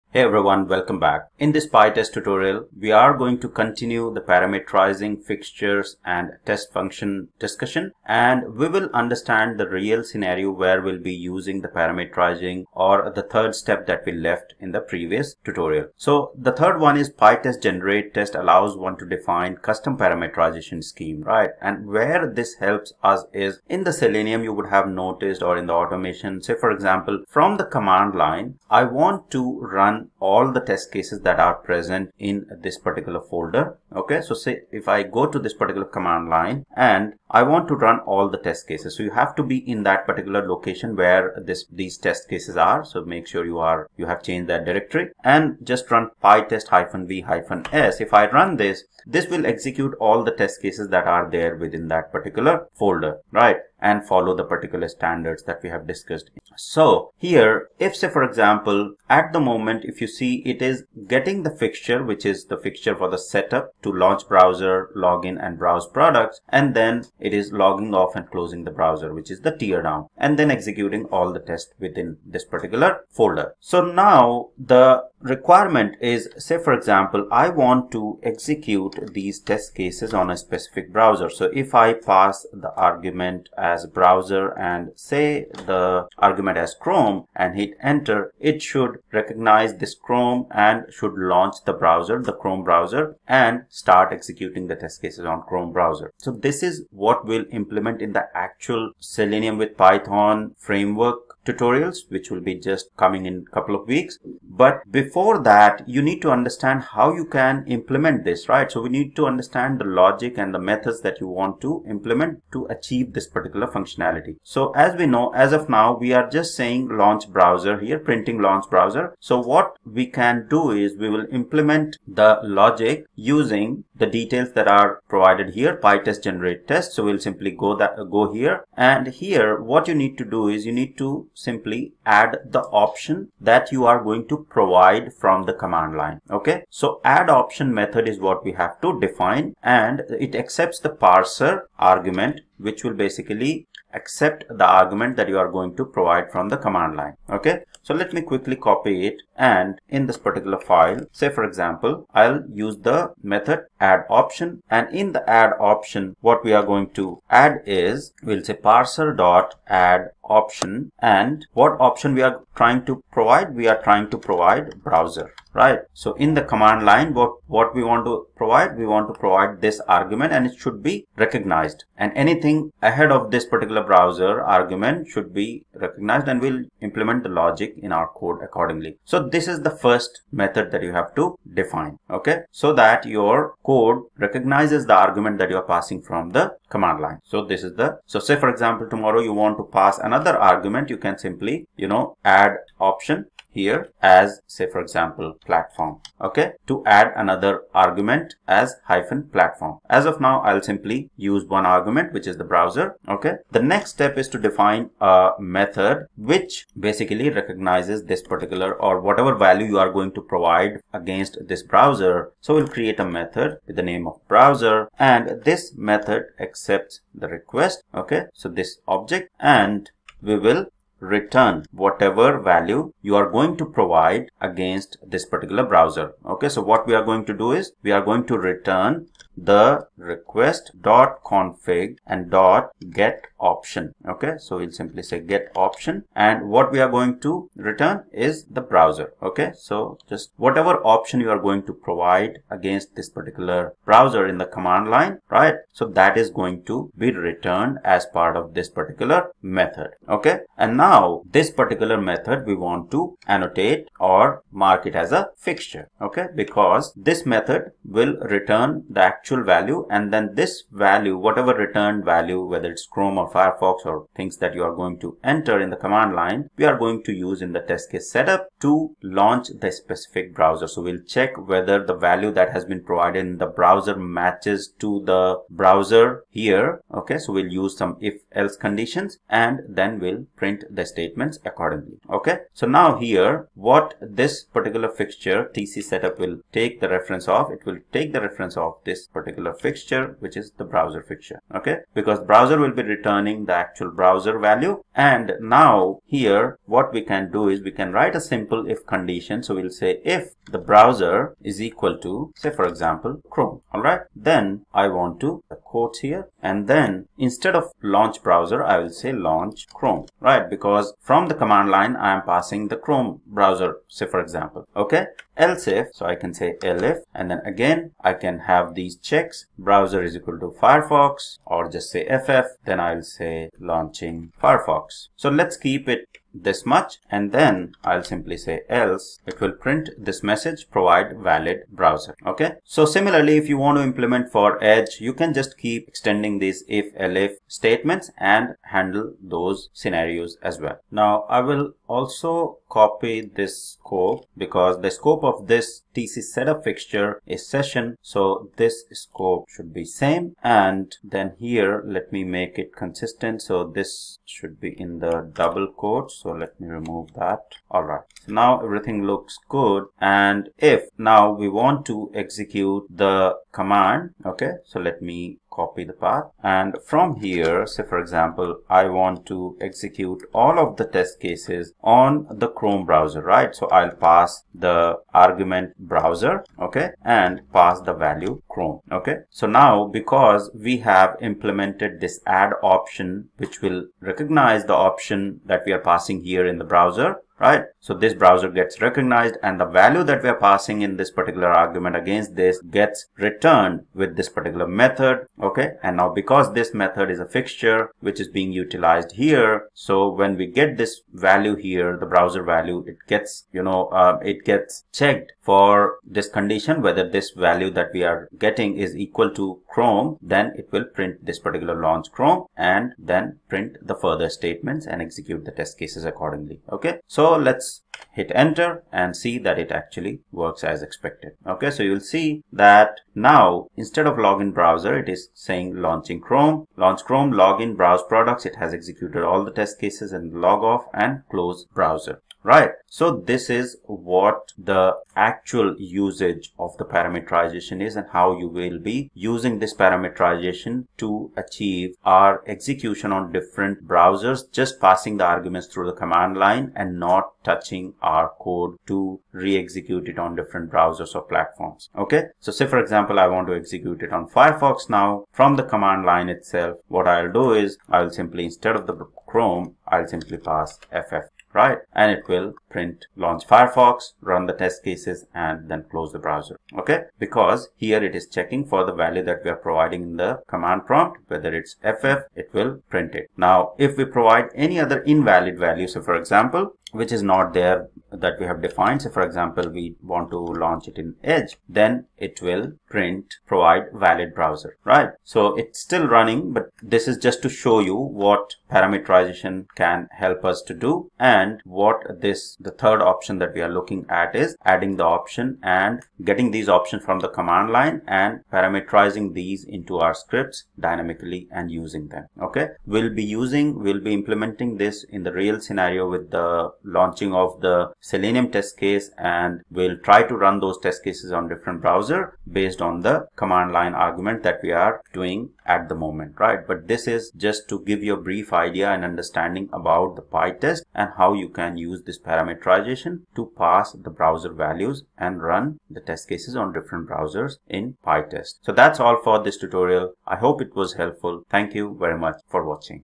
Pytest Tutorial #8 - Parametrizing Fixtures and Pytest Functions- Part 2
In this PyTest Tutorial we will learn parametrizing fixtures and Pytest functions. There are several levels for test parametrization in PyTest and in this video we will learn how to define custom parametrization schemes or extensions.

✅ pytest.fixture() allows one to parametrize fixture functions.
✅ @pytest.mark.parametrize allows one to define multiple sets of arguments and fixtures at the test function or class.
✅ pytest_generate_tests allows one to define custom parametrization schemes or extensions.